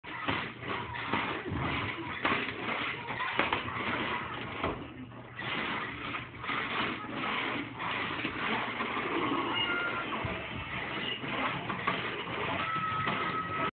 Karamia loves to clean,In a hurry.
My 17 month old Granddaughter, Karamia. I think she was in a little bit of a hurry cleaning this evening.